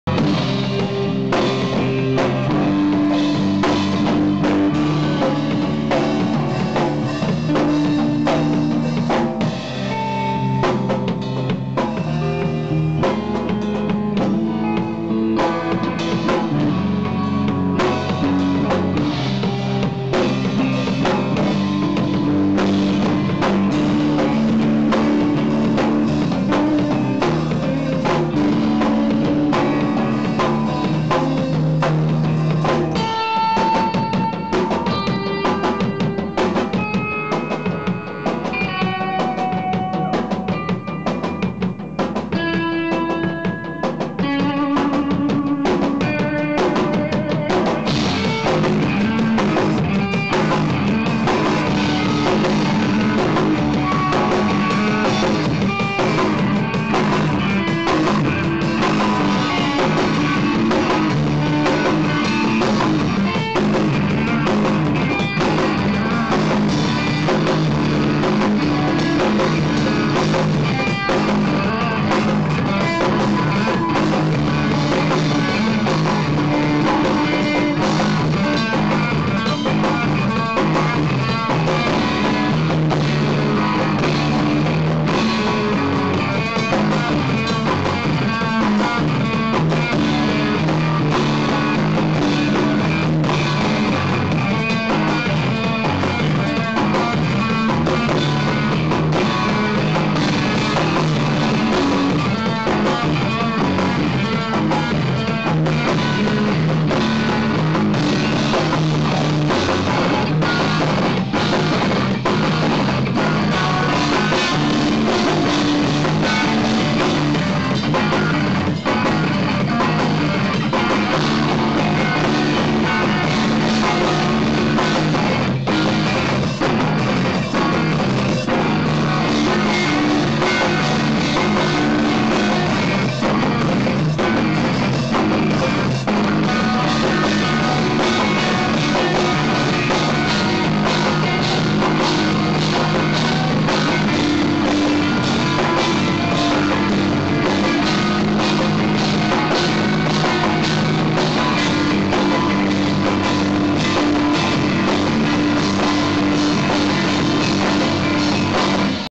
John Makay @ Mojomatic
Live des très bon John Makay au Mojomatic à Montpellier le 16/05/2010